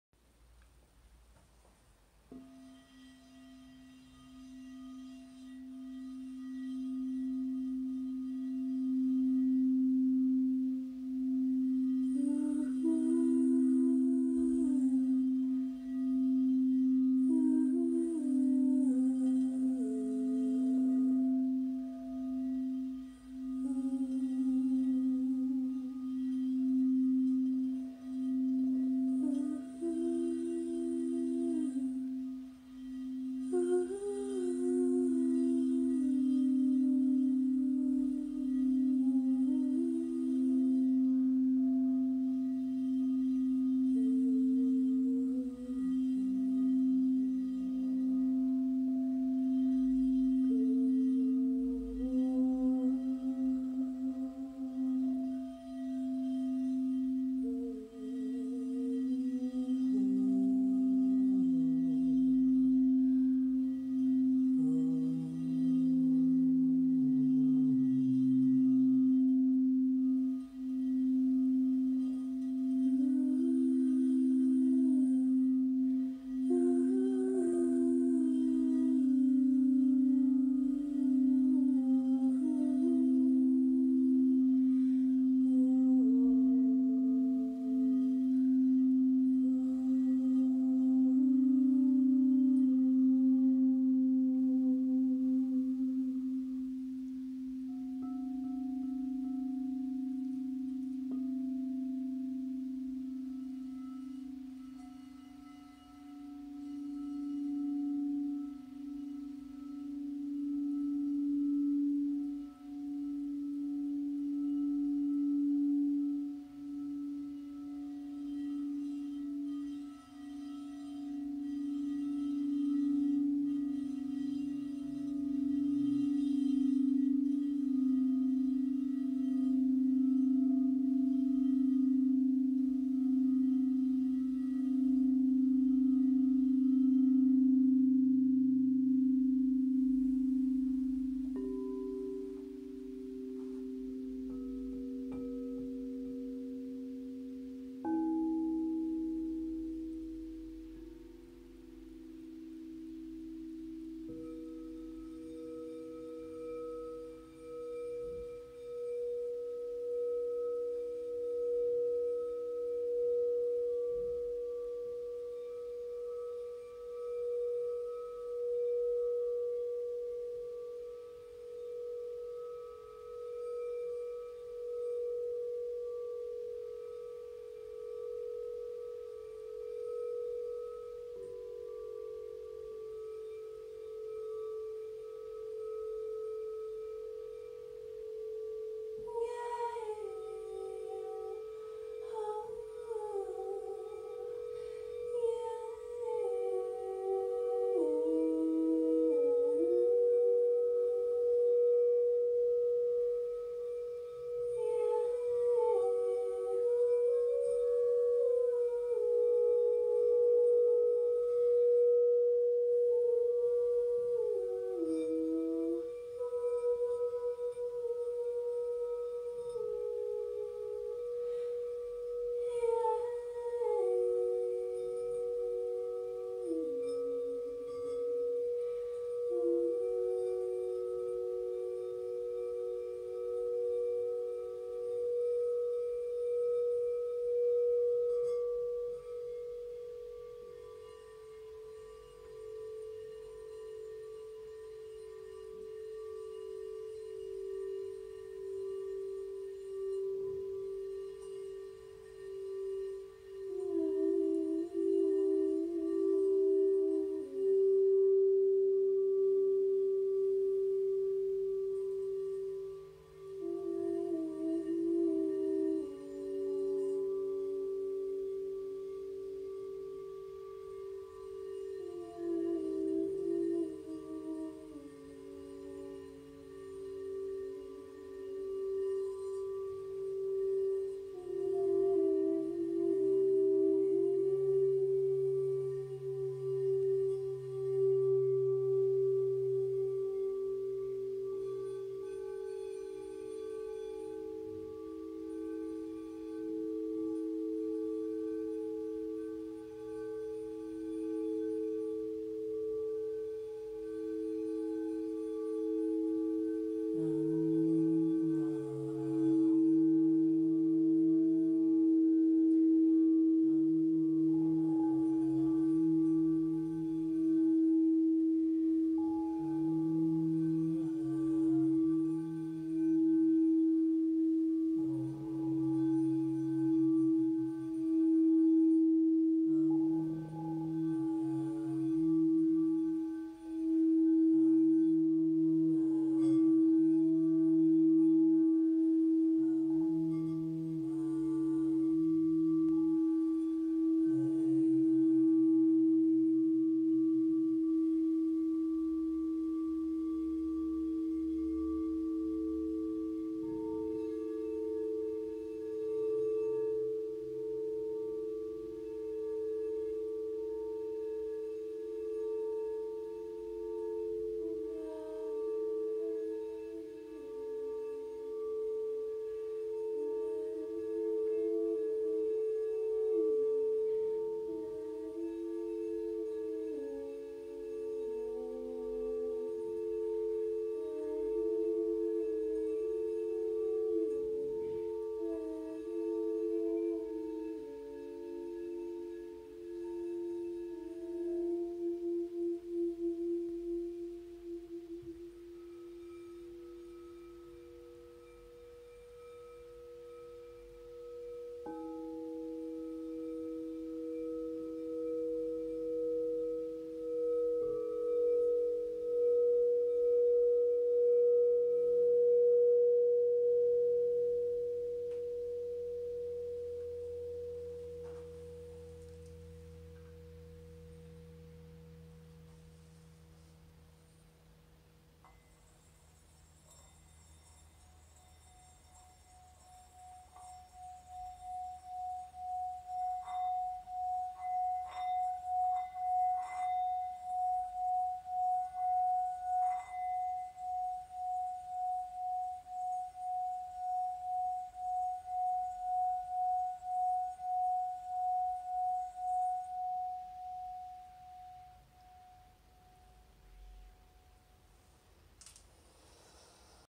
Say Their Names Sound Bath #2
Before I had a chance to post my sound bath dedicated to DaunteWright, AdamToledo was murdered. Now, MakhiaBryant. What is this world coming to? What is happening in our country? I hope this video brings you peace. Singing with my singing bowls always helps me tremendously in moments of overwhelm. And so I share.